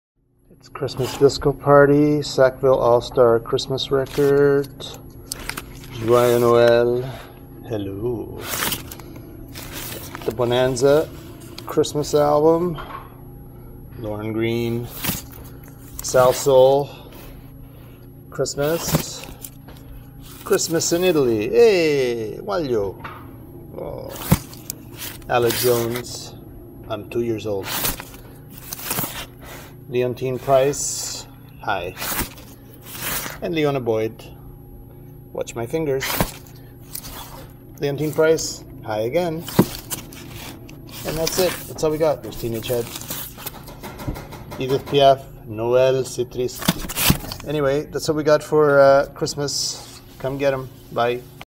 BBAM! Gallery 2016 Vinyl LPs Sale
We have a slim selection of Christmas vinyl this year. Come have a look!